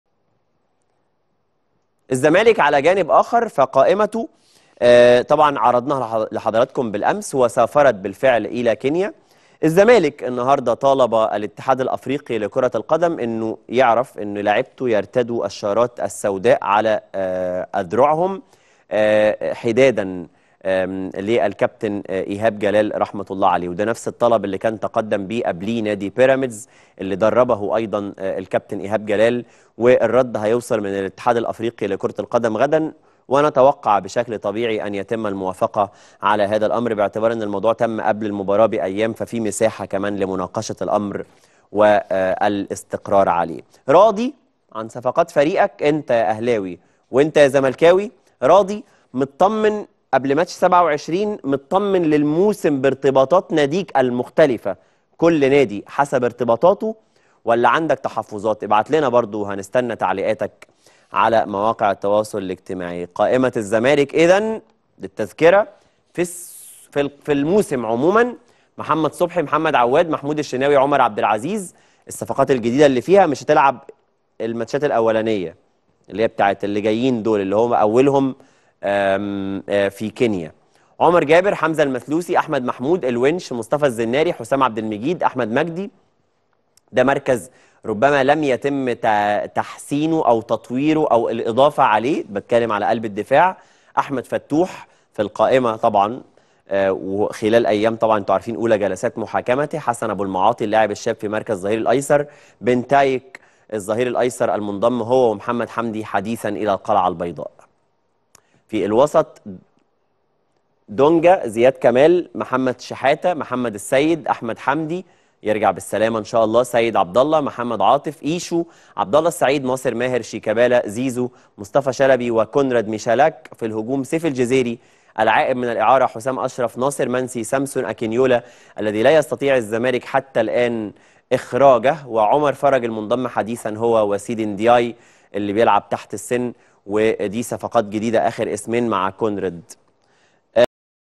قائمة الزمالك الأفريقية للموسم الجديد
جزء من حلقة اليوم 12 سبتمبر 2024 مع هاني حتحوت وفيها نعي الراحل ايهاب جلال ،وتصريحات محمد عبدالمنعم ،والزمالك يعلن تعاقده مع البولندي كونراد ميشالاك ،وقائمة الأهلي لمباراة السوبر ،وقائمة الزمالك الأفريقية للموسم الجديد ،ولقاء مع هاني العقبي يتحدث عن الكابتن ايهاب جلال ويناقش اهم اخبار الكرة.